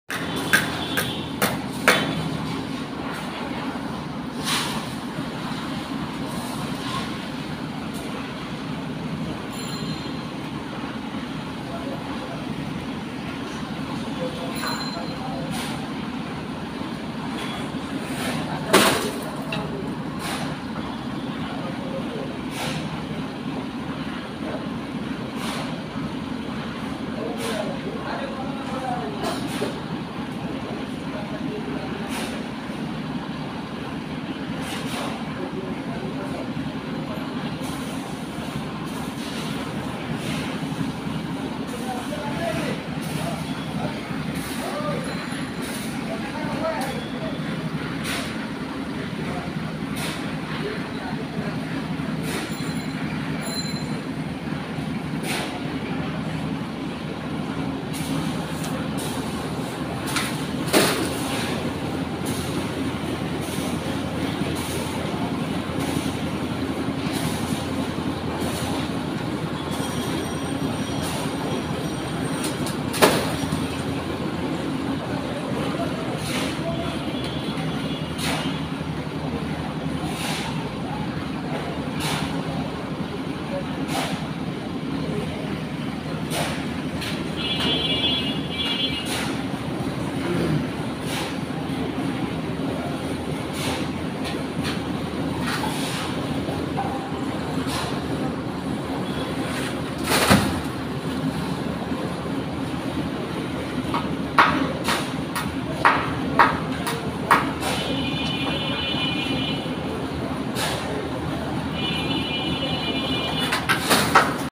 কি কষ্টের কাজ। কারো সুখের জন্য কত মানুষের কষ্ট। # worker at mid night#
কত রিস্কের কাজ। একটু অসচেতনতা বশত হতে পারে অনেক বড় দূর্ঘটনা।